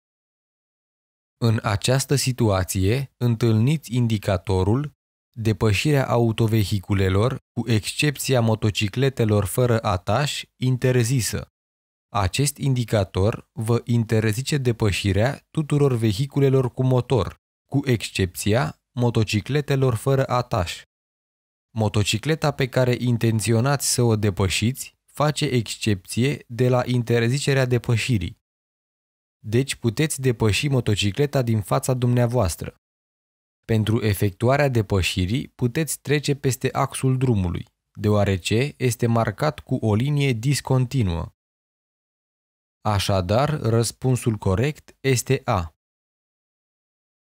Este corect să depăşeşti motocicleta din faţa ta?
A. da;
B. nu, deoarece astfel vei depăşi axul drumului;
C. nu, deoarece nu respecţi semnificaţia indicatorului.